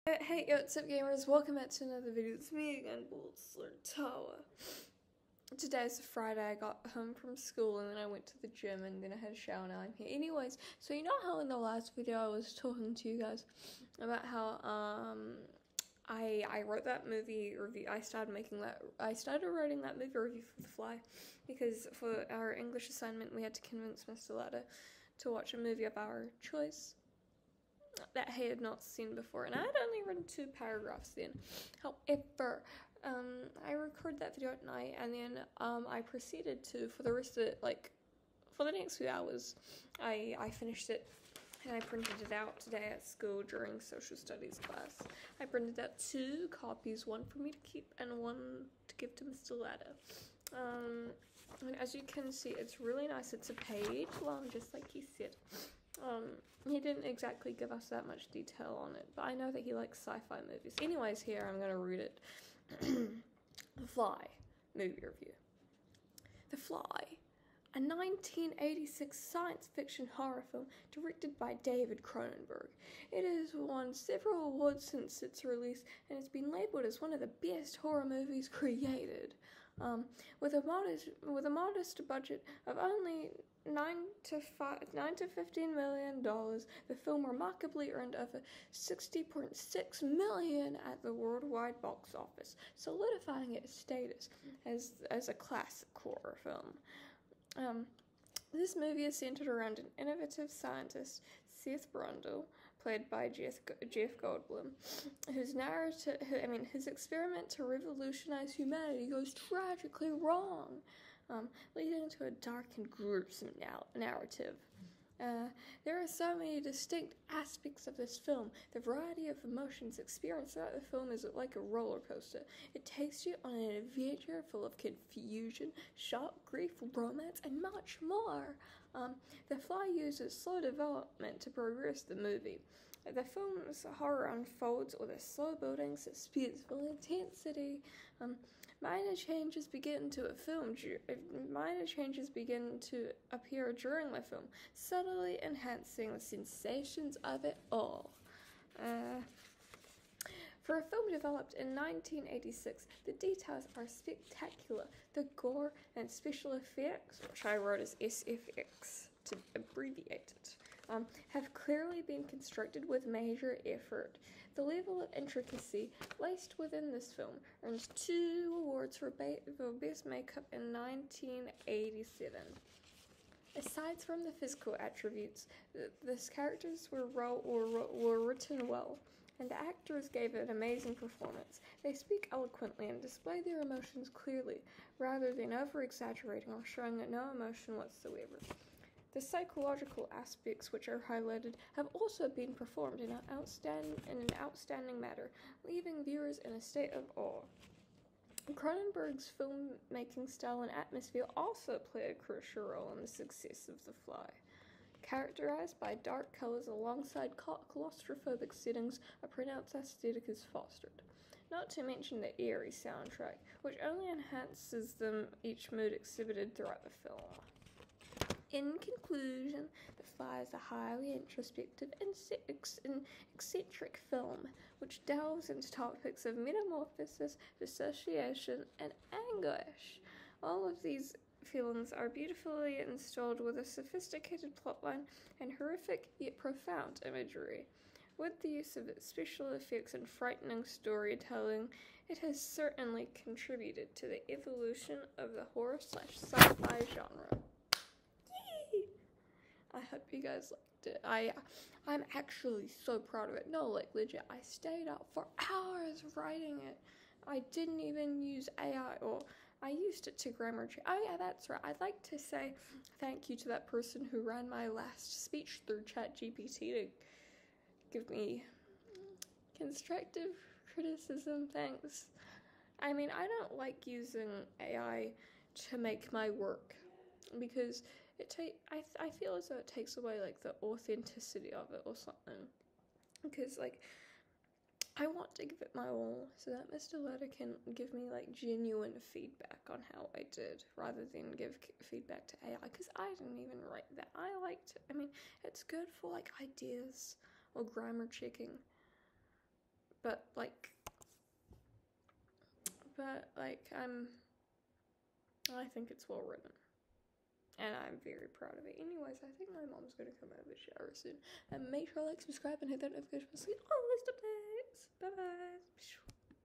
The Fly - movie review by tawa
i worked very hard on this opinion writing piece! no spoilers! just FACTS!!

🪰🎃 MY LINKS: 🎃🪰
MY COOL PORTAL THAT I CODED!